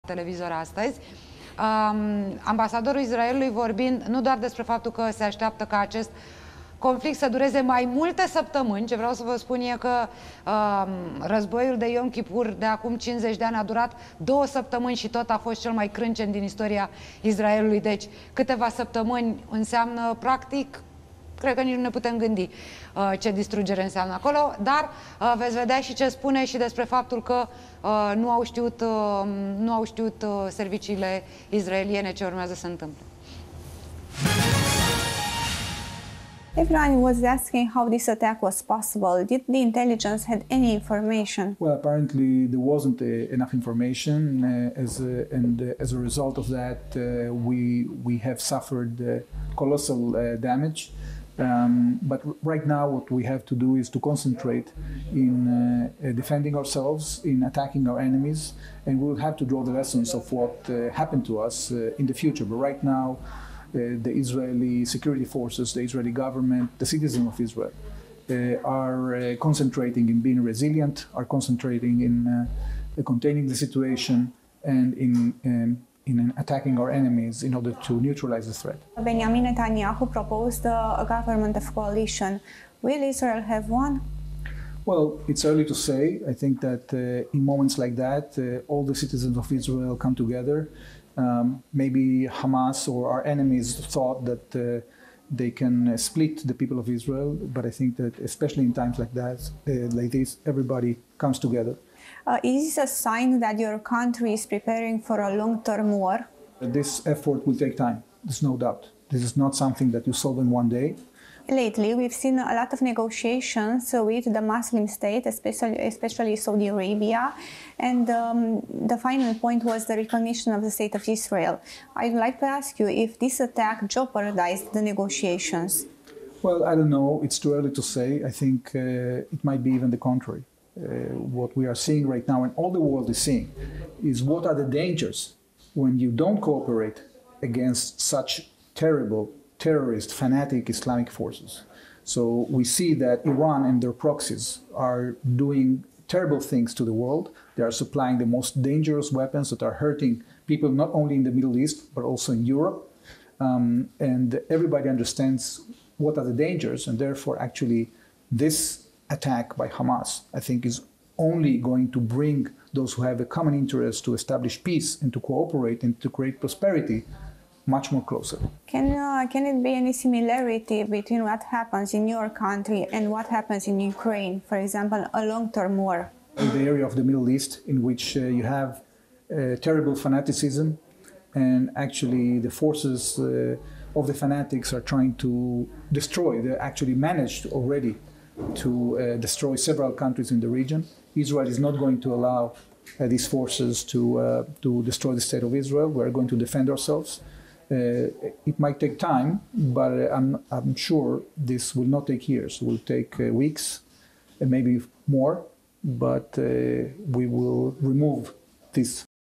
Reuven Azar, ambasadorul Israelului la Bucureşti: Se pare că nu au existat suficiente informaţii
Reuven Azar, ambasadorul Israelului la Bucureşti, a declarat, într-un interviu exclusiv pentru Antena 3 CNN, că nu au existat suficiente informaţii despre războiul declanşat de luptăorii Hamas în Israel.